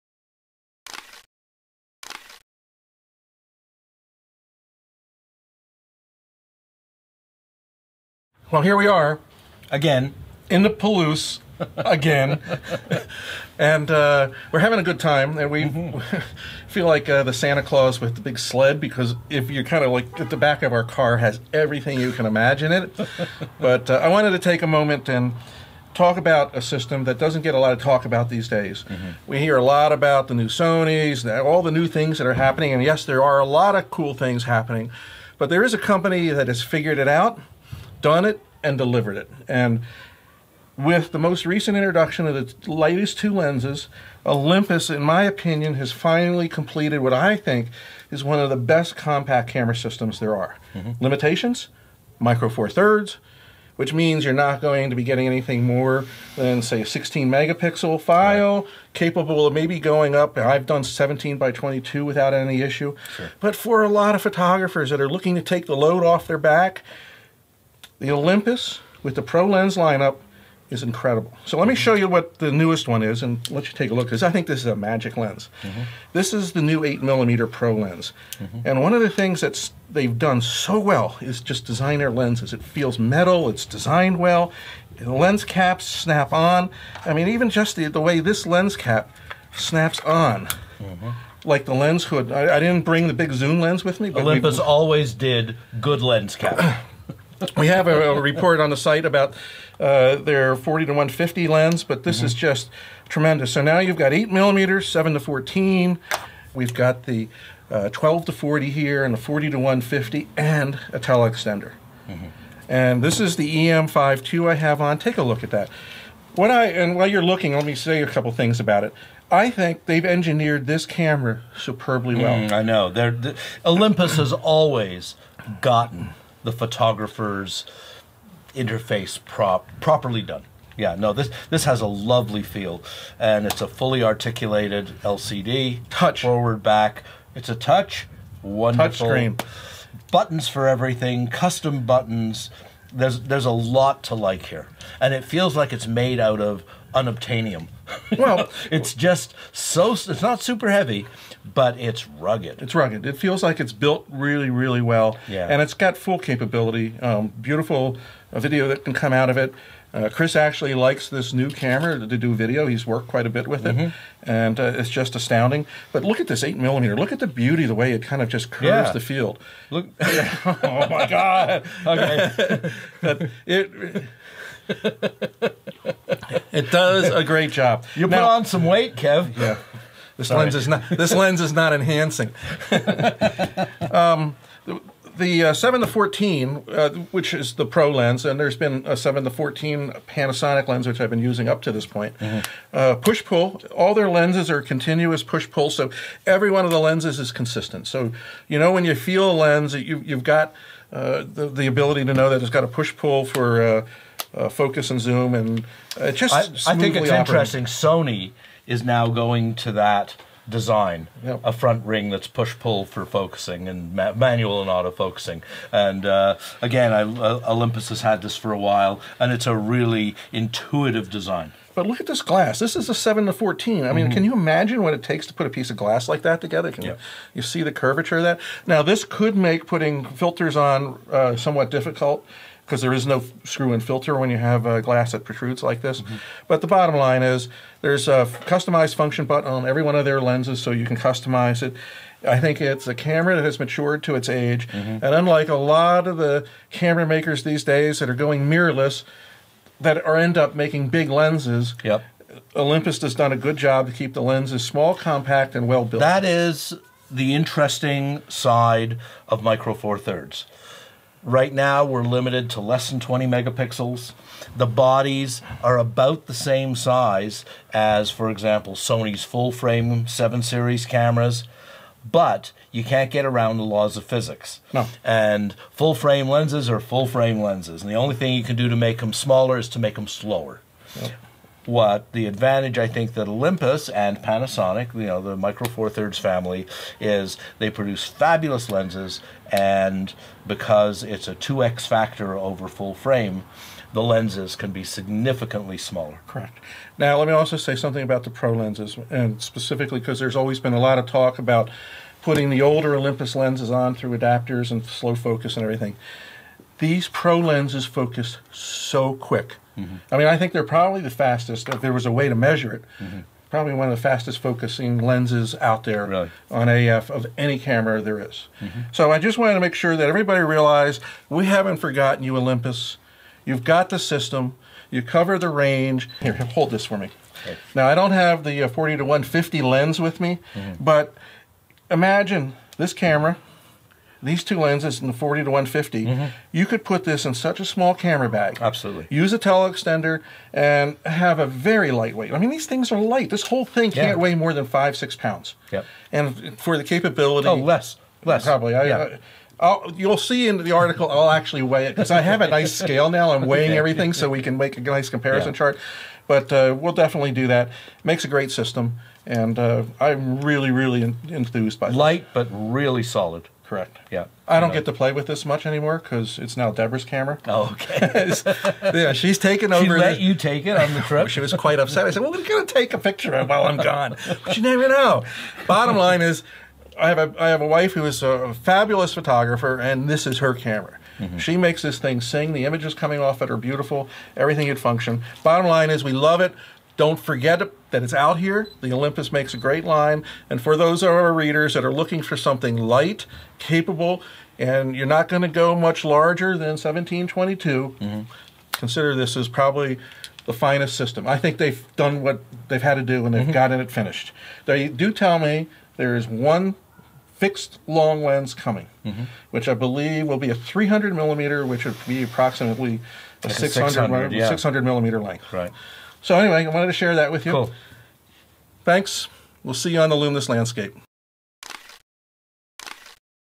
OLYMPUS OM D E M5 ll system
Kevin Raber looks at the Olympus OM-D  E-M5 ll system and its associated Pro lens lineup. This is now a fully mature and exceptional micro four-thirds system.